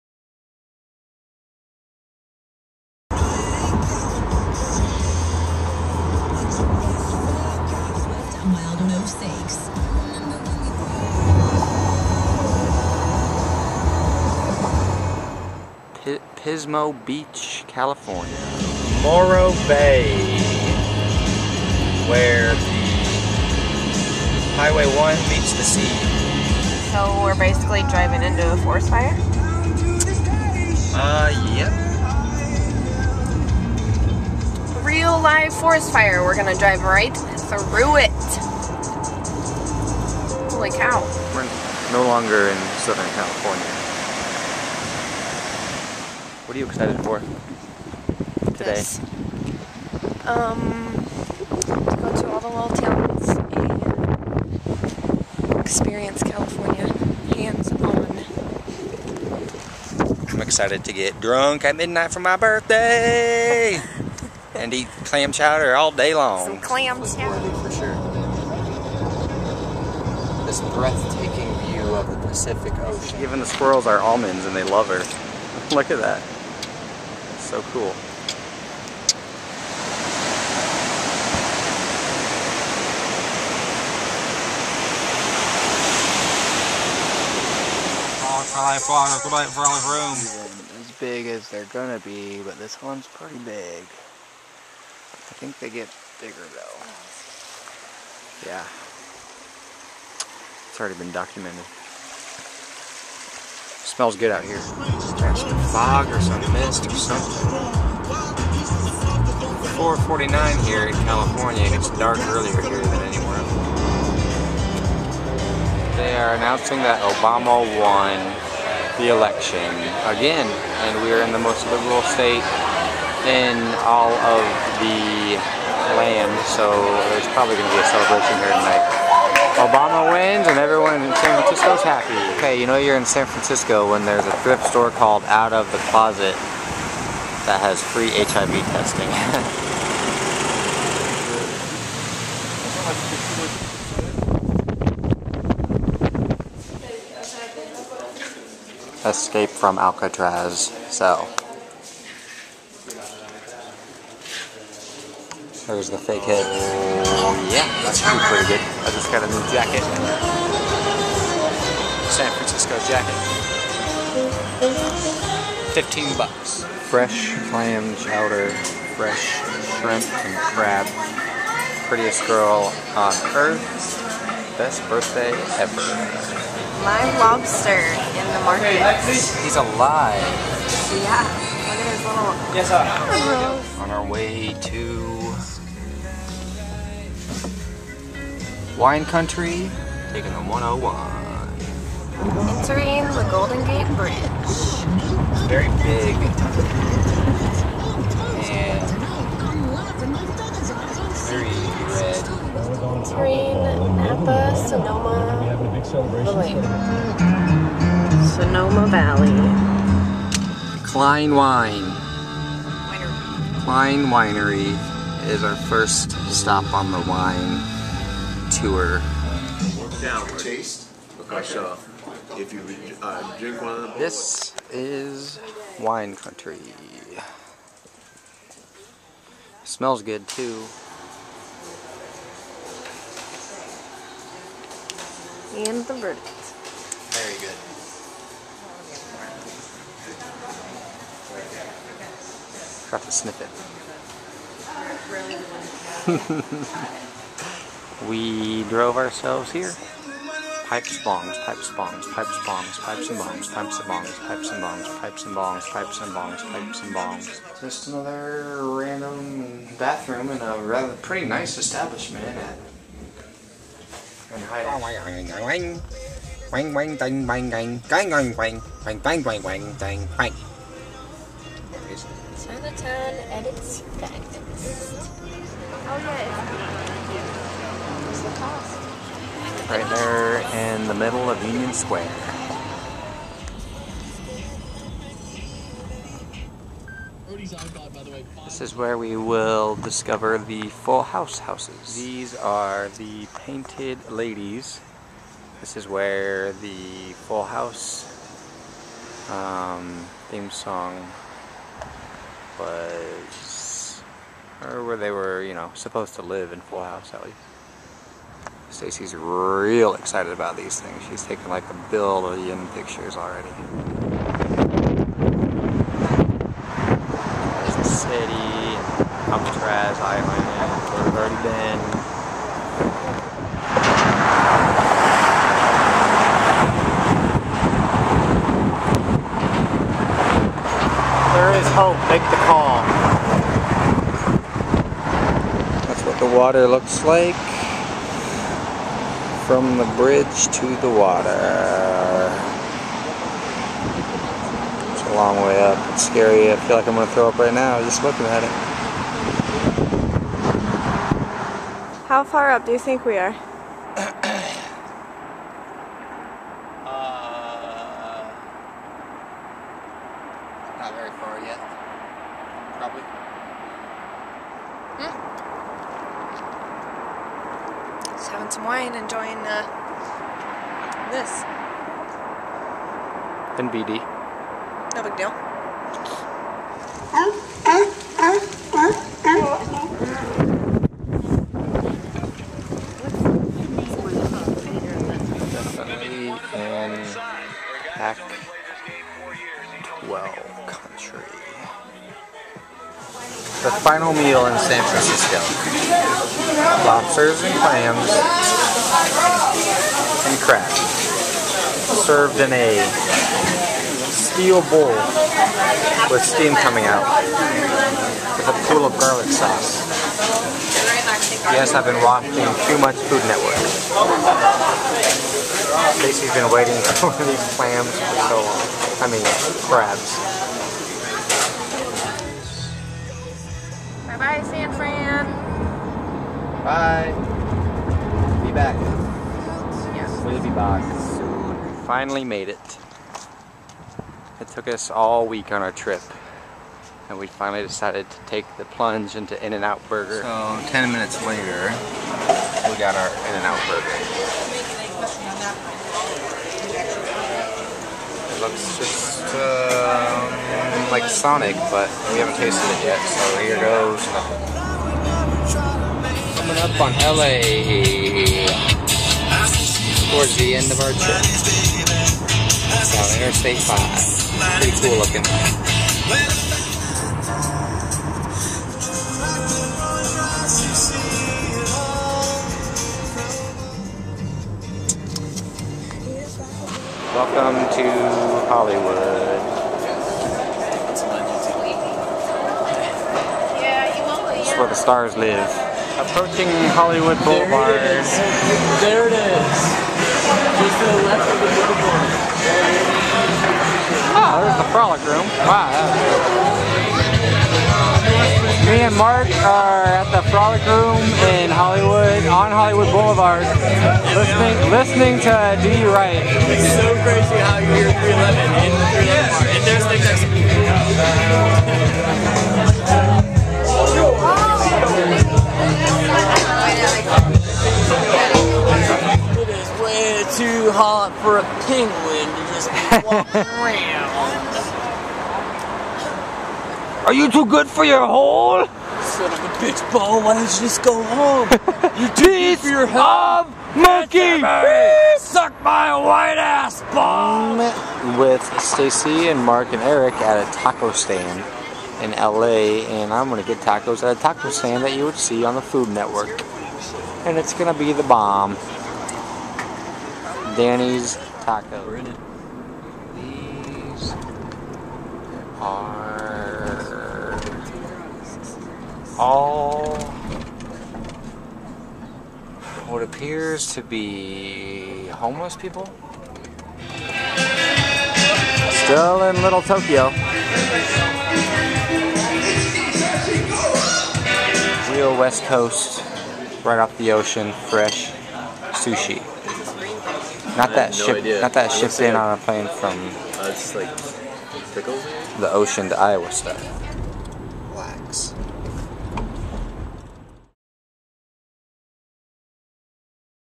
Staci and TJ go to California
My girlfriend and I flew into L.A. drove up the coast to San Francisco to hang, and then it was back to L.A. to rendezvous with a friend.